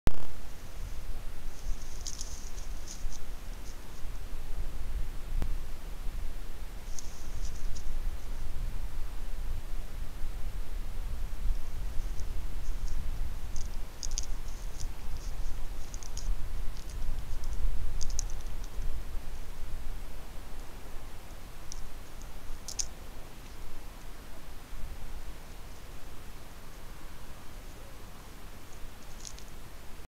Chat vs renard...
Admirez le comportement de l'un et de l'autre...
Le renard semble à la fois détaché mais intéressé. Il passe du détendu au joueur excité puis urine et attrape une branche sûrement par frustration.
Le chat, fidèle à lui même, observe et fait le dos rond, histoire de bluffer un peu le prétendant à la bonne chair...
Résultat, chacun part de son côté ; match nul !